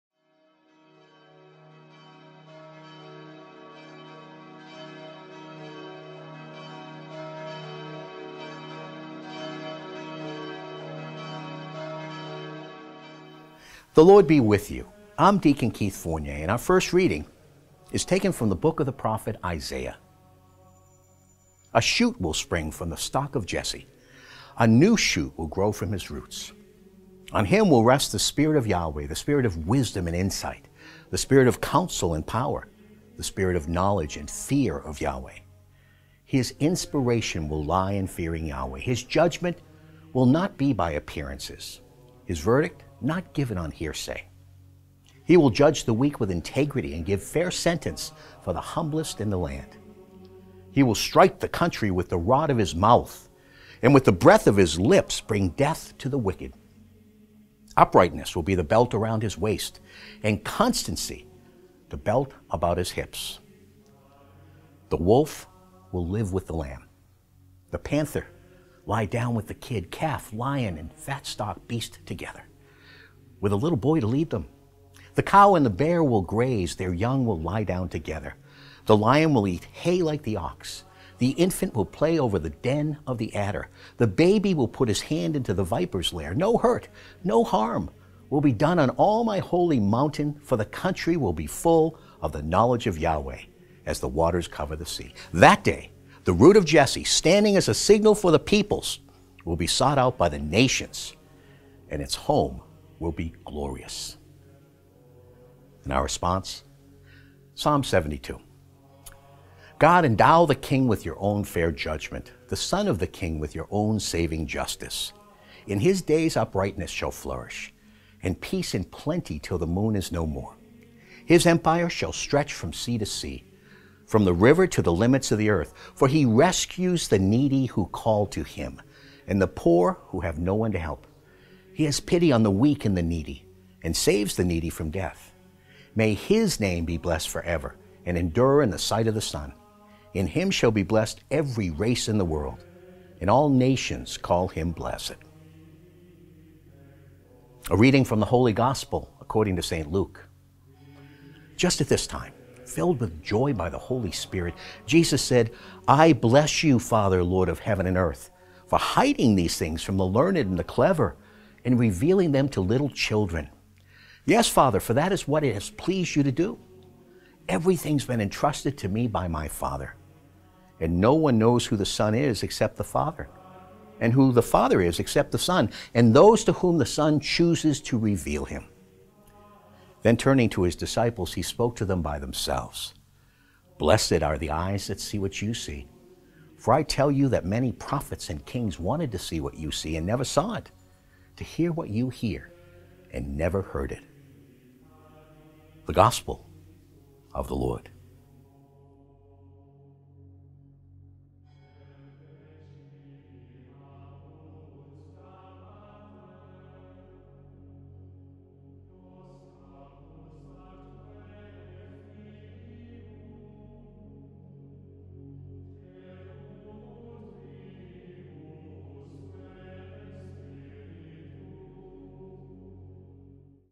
Daily Reading for Tuesday, December 3rd, 2019 HD
1 A shoot will spring from the stock of Jesse, a new shoot will grow from his roots.

2 On him will rest the spirit of Yahweh, the spirit of wisdom and insight, the spirit of counsel and power, the spirit of knowledge and fear of Yahweh:

3 his inspiration will lie in fearing Yahweh. His judgement will not be by appearances. his verdict not given on hearsay.

4 He will judge the weak with integrity and give fair sentence for the humblest in the land. He will strike the country with the rod of his mouth and with the breath of his lips bring death to the wicked.

5 Uprightness will be the belt around his waist, and constancy the belt about his hips.

6 The wolf will live with the lamb, the panther lie down with the kid, calf, lion and fat-stock beast together, with a little boy to lead them.

7 The cow and the bear will graze, their young will lie down together. The lion will eat hay like the ox.

8 The infant will play over the den of the adder; the baby will put his hand into the viper's lair.

9 No hurt, no harm will be done on all my holy mountain, for the country will be full of knowledge of Yahweh as the waters cover the sea.

10 That day, the root of Jesse, standing as a signal for the peoples, will be sought out by the nations and its home will be glorious.

1 [Of Solomon] God, endow the king with your own fair judgement, the son of the king with your own saving justice,

7 In his days uprightness shall flourish, and peace in plenty till the moon is no more.

8 His empire shall stretch from sea to sea, from the river to the limits of the earth.

12 For he rescues the needy who calls to him, and the poor who has no one to help.

13 He has pity on the weak and the needy, and saves the needy from death.

17 May his name be blessed for ever, and endure in the sight of the sun. In him shall be blessed every race in the world, and all nations call him blessed.

21 Just at this time, filled with joy by the Holy Spirit, he said, 'I bless you, Father, Lord of heaven and of earth, for hiding these things from the learned and the clever and revealing them to little children. Yes, Father, for that is what it has pleased you to do.

22 Everything has been entrusted to me by my Father; and no one knows who the Son is except the Father, and who the Father is except the Son and those to whom the Son chooses to reveal him.'

23 Then turning to his disciples he spoke to them by themselves, 'Blessed are the eyes that see what you see,

24 for I tell you that many prophets and kings wanted to see what you see, and never saw it; to hear what you hear, and never heard it.'